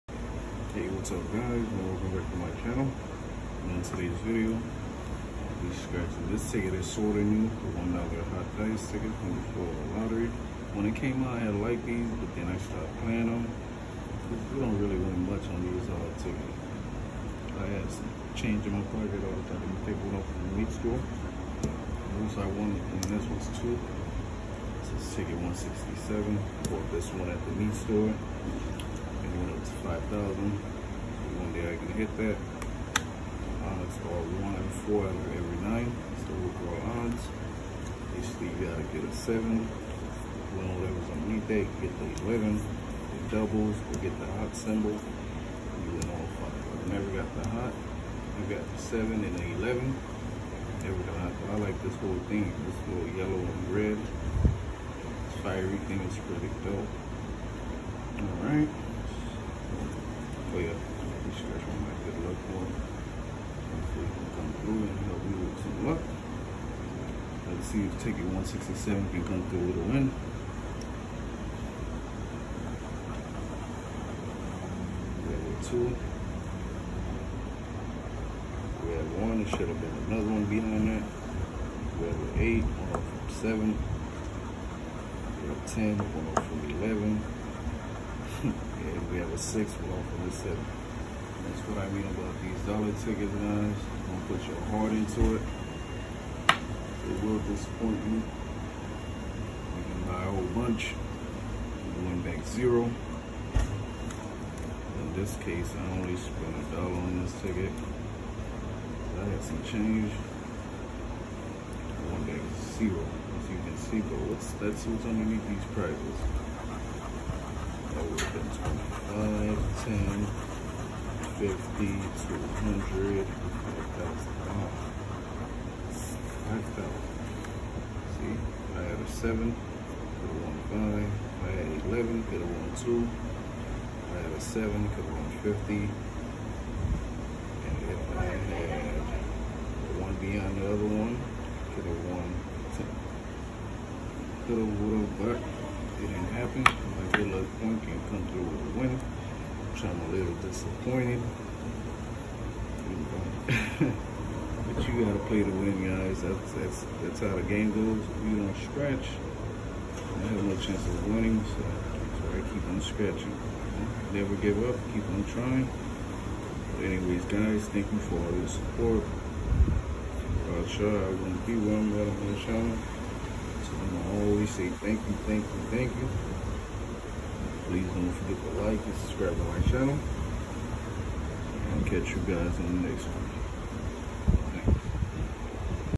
$1 HOT DICE SCRATCH-OFFS
Spent $1 on a Hot dice scratchoffs from the florida lottery